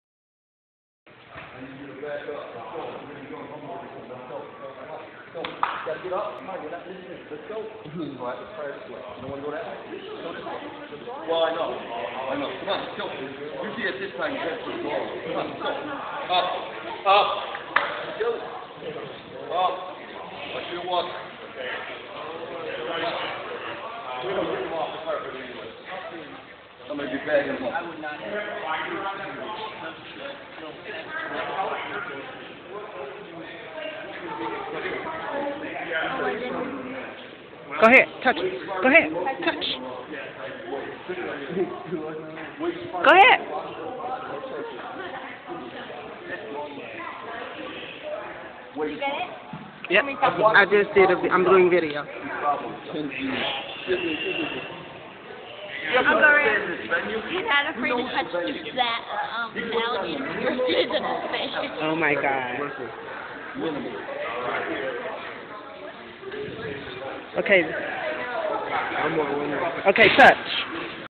Bubba the alligator goes home!
Trainer says "UP, let's go home!!" & Bubba went into his carrier bag! I can hardly believe it! LOL